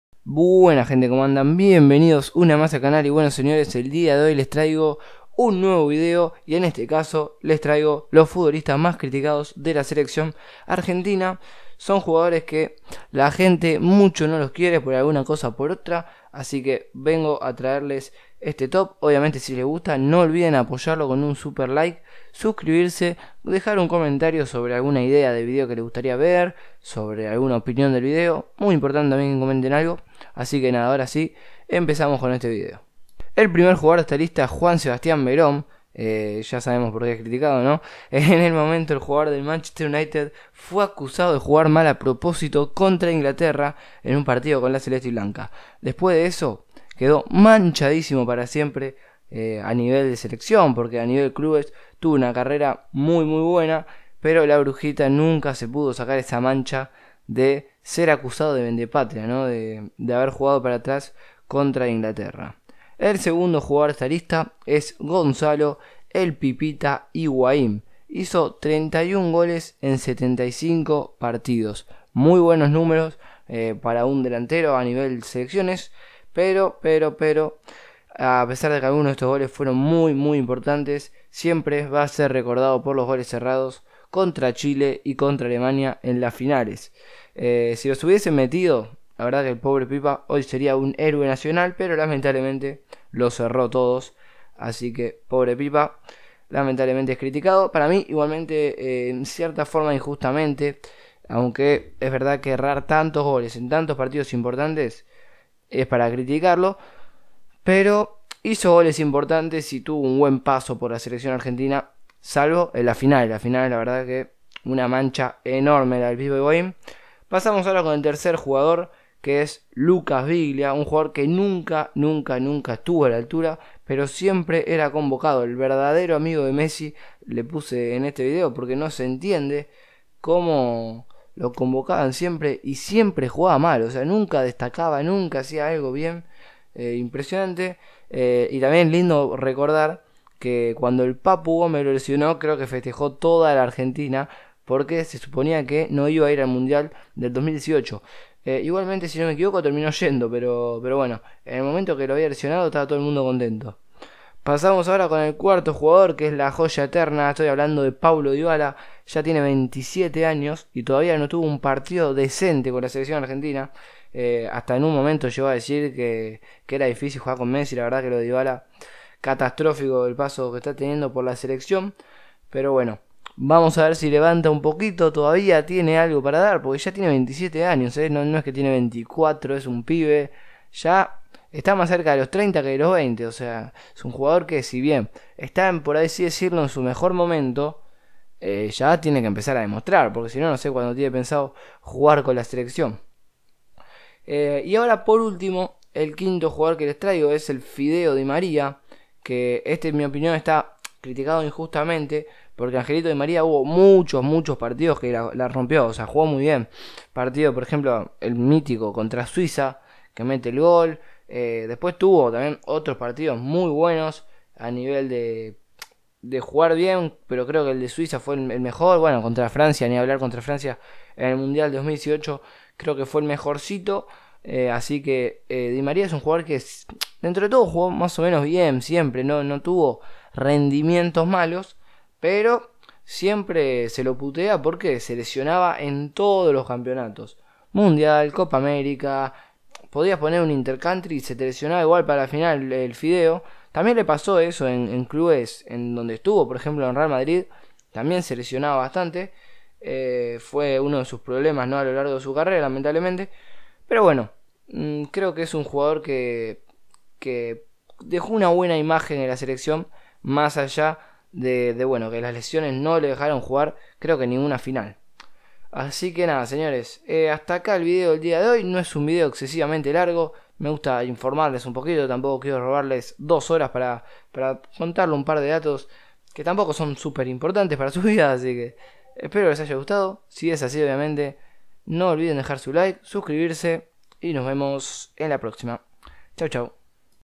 LOS JUGADORES MAS CRITICADOS DE LA SELECCION ARGENTINA
Buenas el día de hoy les traigo los 5 jugadores mas odiados por los hinchas argentinos. ya sea por malos, por ser los peores o por errar goles.  Veron por vende patria, Higuain y sus erradas, dybala la joya eterna, biglia vs papu gomez, y di maria criticado en mi opinion injustamente porque tuvo muy buenos partidos no nos olvidemos del gol a suiza, etc